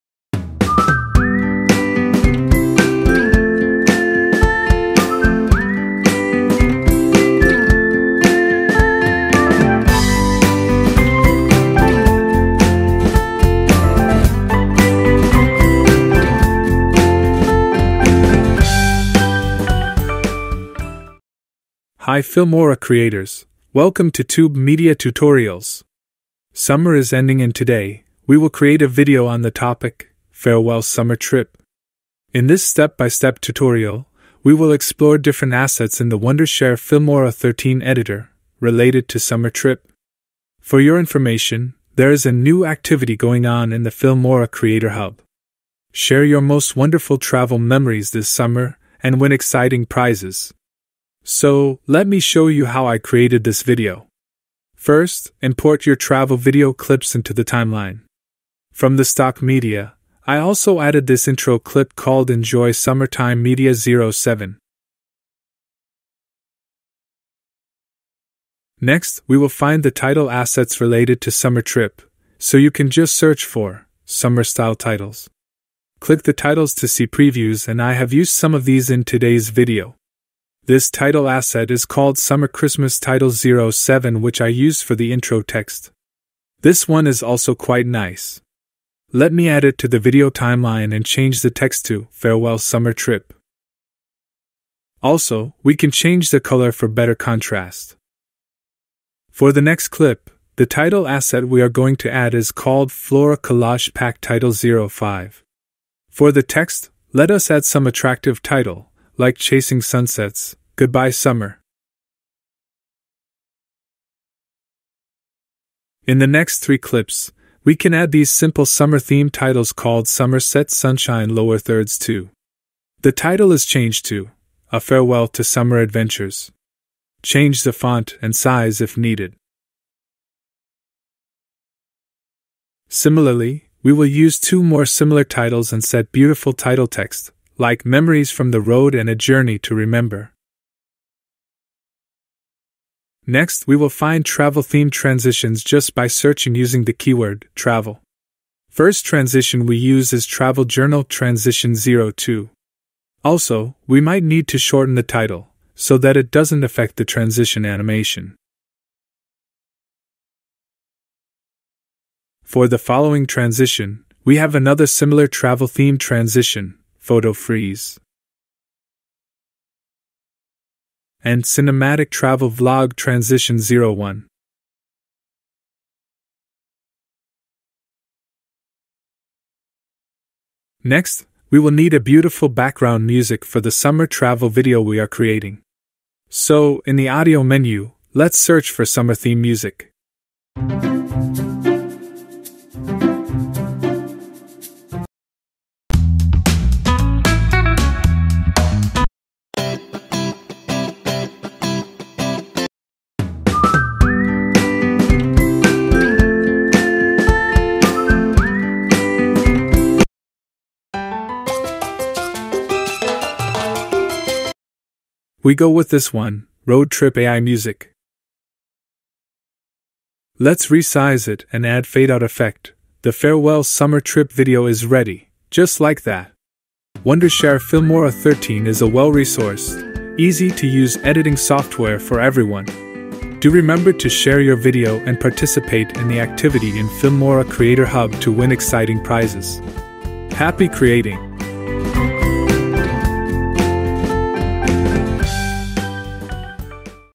Create Stunning 'Farewell Summer Trip' Videos using Filmora 13! Complete Guide
Participate in the 'Farewell Summer Trip' video campaign in the creator hub of Filmora 13 and win exciting prizes.